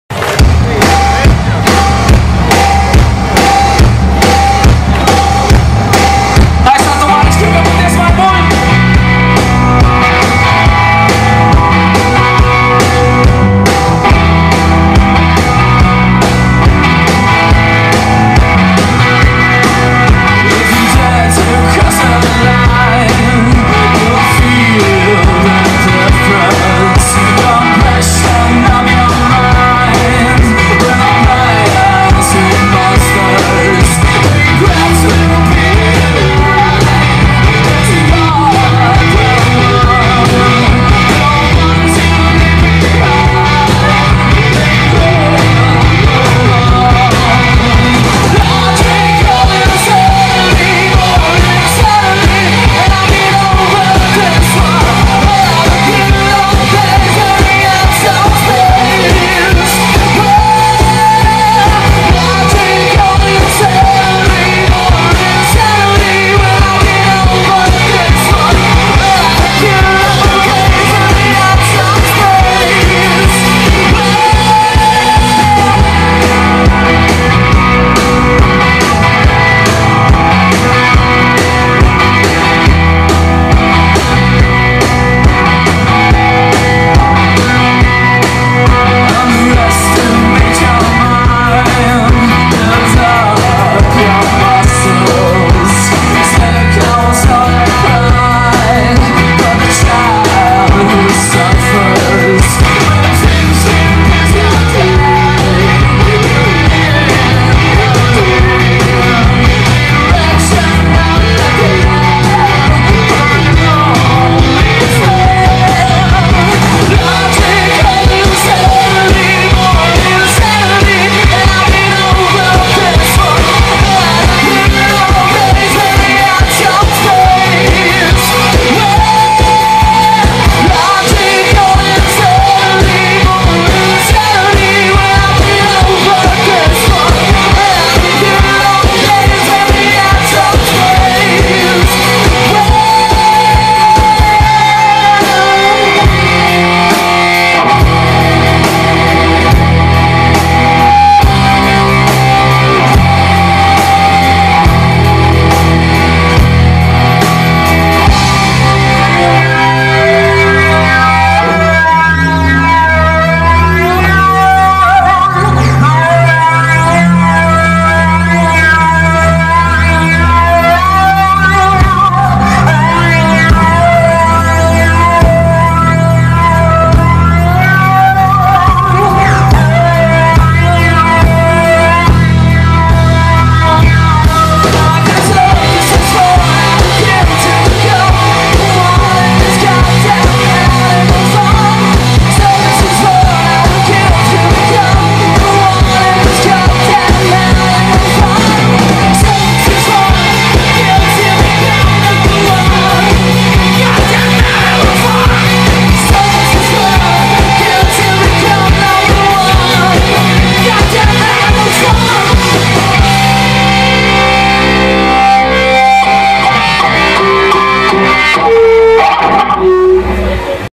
Grimus @ Samfest Rock 2008
Grimus Live @ Samfest Rock 2008, Satu Mare, Romania
Copyright Samfest Rock Romania and "GM Zamfirescu" Cultural Center